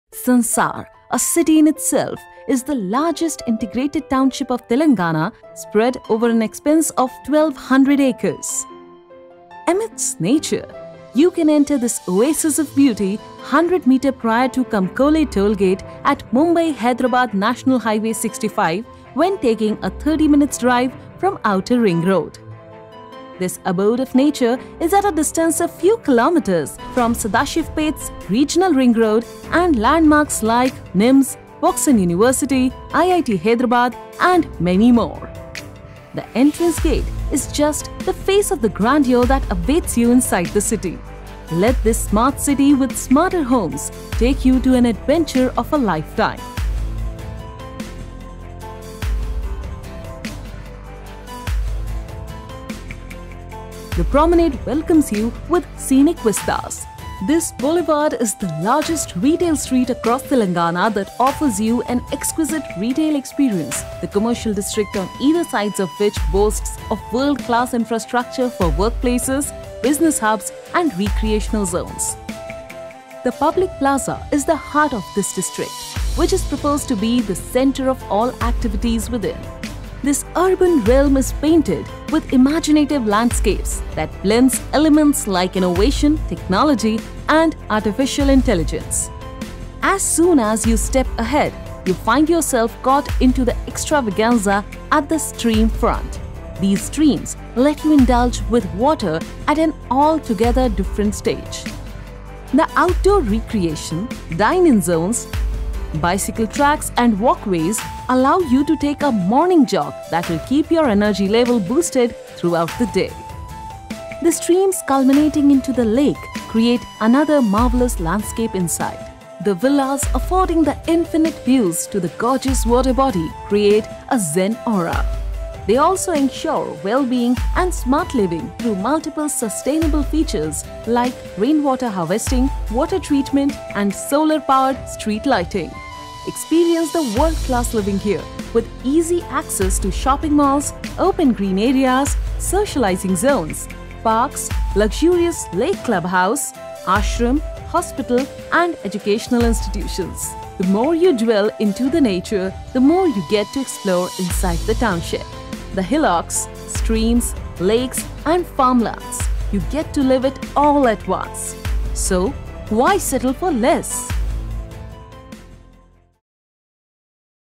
Atriya Sansaar - A Universe of Possibilities
Proudly positioning itself as ‘the largest township in Telangana State’, Sansaar is the gateway to a world of premium lifestyle and reimagined living. Be a part of the future.